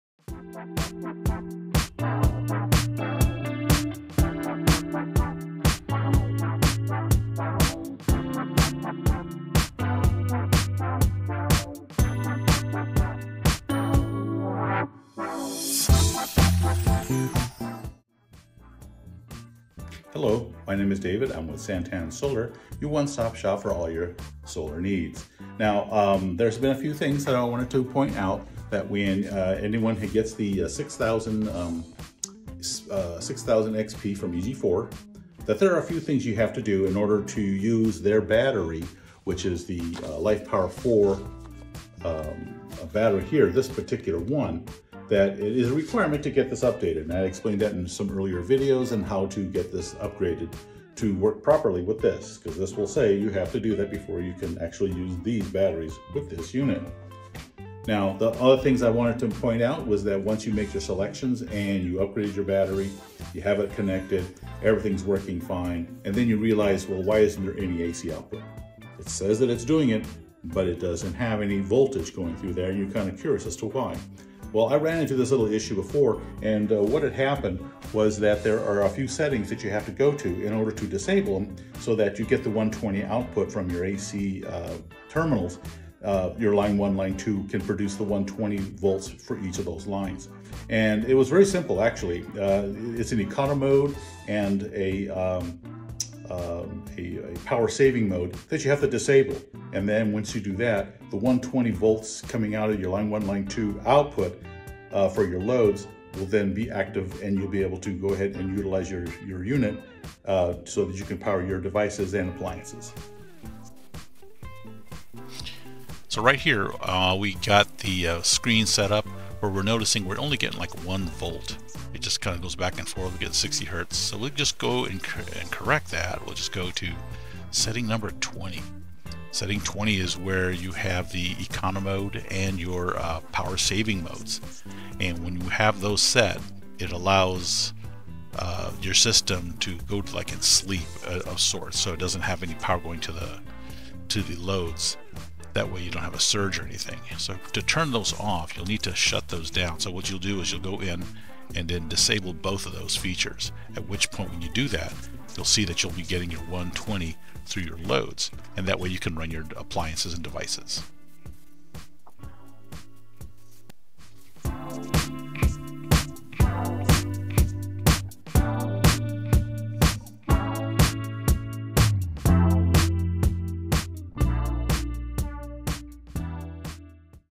EG4 6000XP Battery - Output Tips
Part 3 of getting to know your EG4 battery! We want your experience with our products to be flawless. Follow our tips and tricks for the best results!

Visit SanTanSolar.com